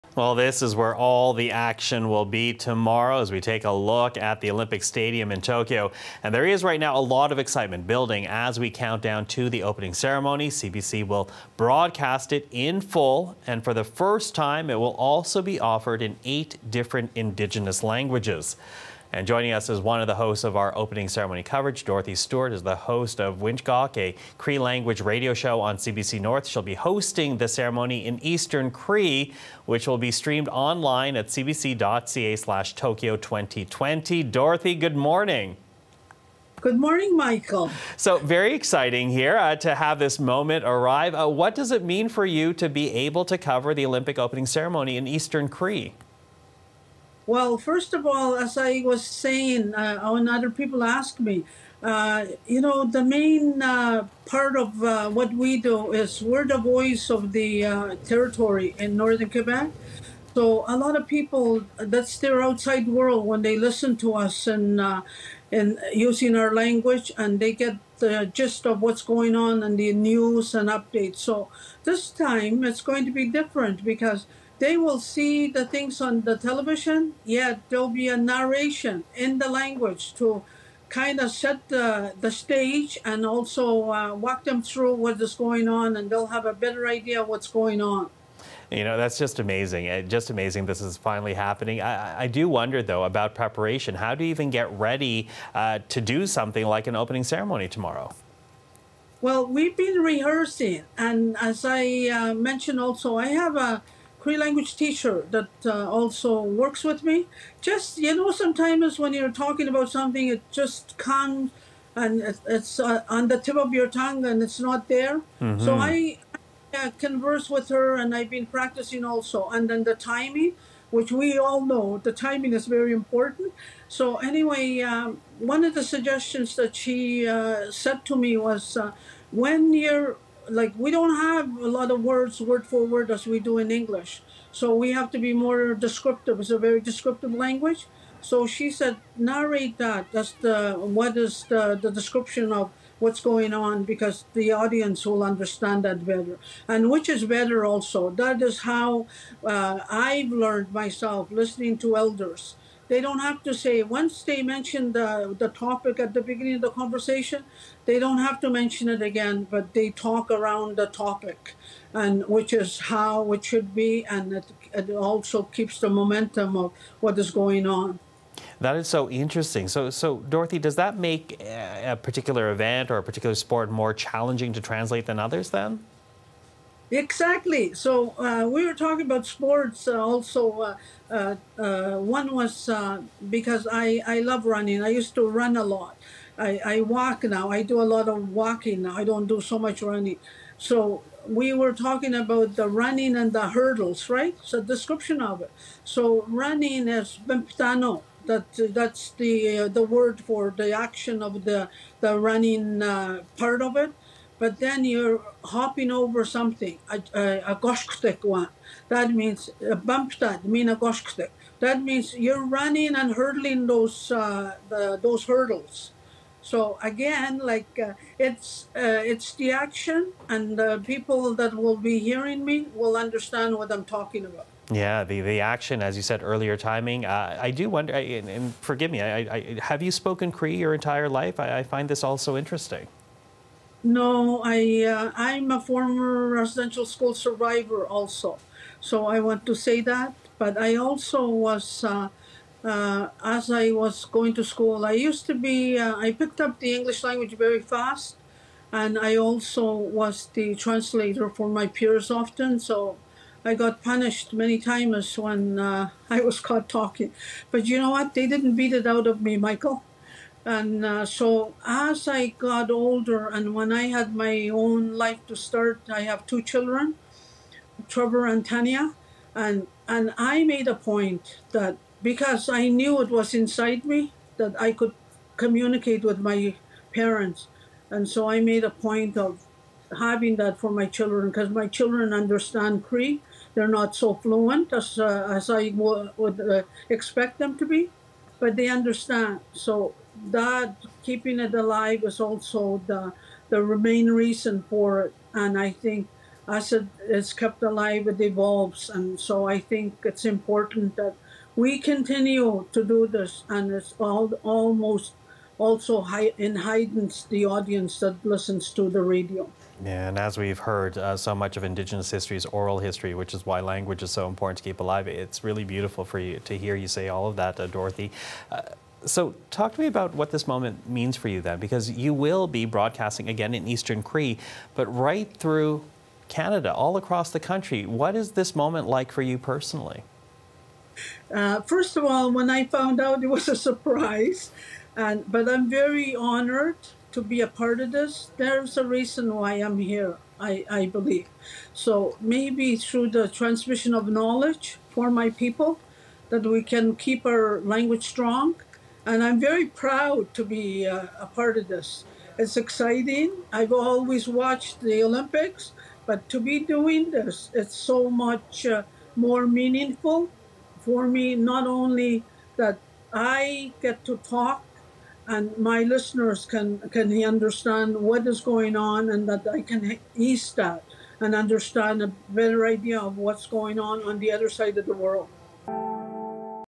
What it's like translating Tokyo 2020 coverage into Eastern Cree
Dorothy Stewart, host of the CBC North radio show Winschgaoug, says she is proud to be translating the Tokyo Olympics opening ceremony into Eastern Cree. This year, CBC will provide live coverage in eight different Indigenous languages that will be streamed online.